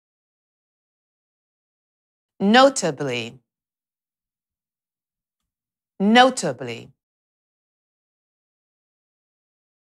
How to pronounce NOTABLY in American English
This video shows you how to pronounce NOTABLY in American English. Speaker has an accent from Houston, Texas. Collins Dictionary, the home of living English and pioneers of dictionary publishing